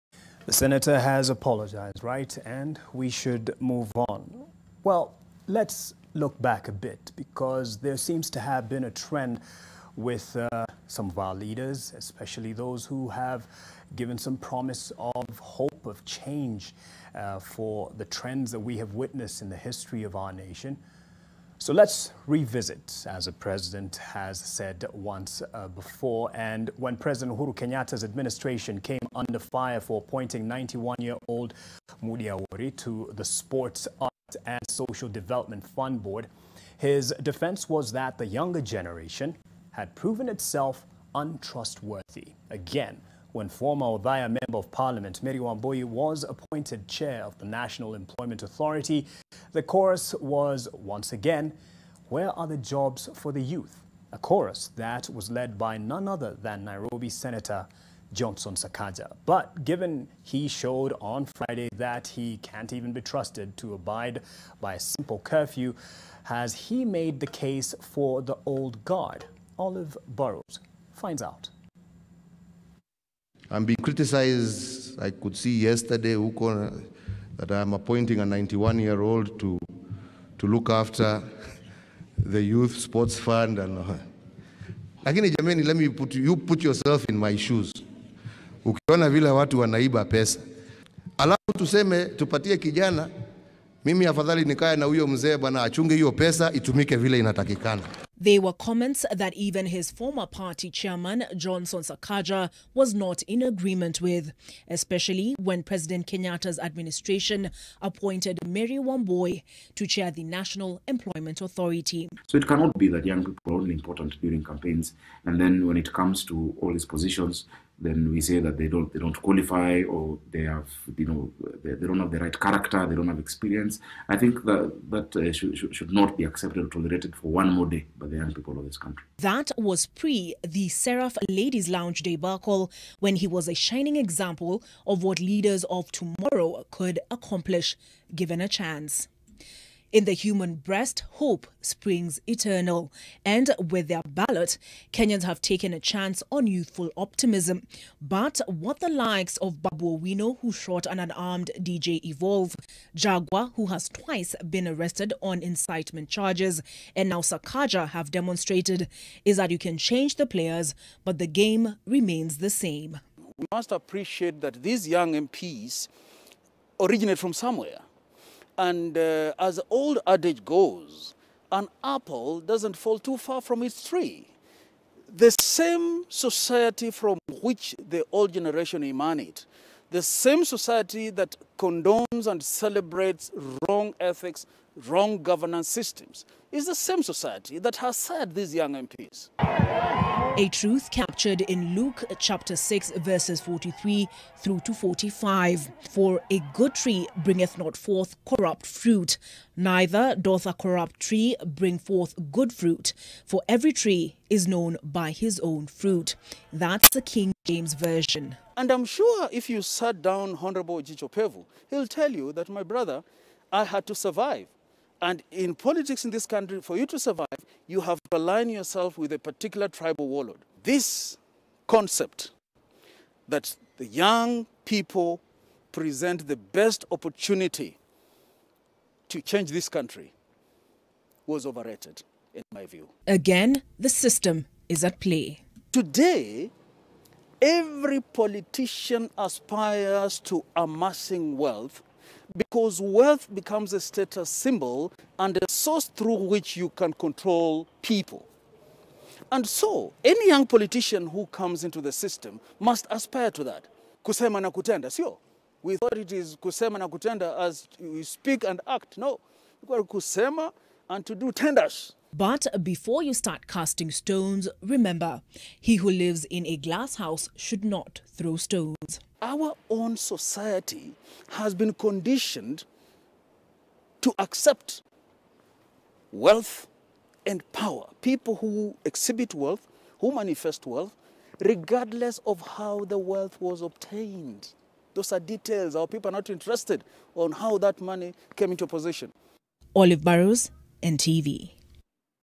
Young leaders failing Kenyans? Was President Uhuru right?
When President Uhuru Kenyatta's administration came under fire for appointing 91-year-old Moody Awori to the Sports, Art and Social Development Fund board, his defence was that the younger generation had proven itself untrustworthy could this be true?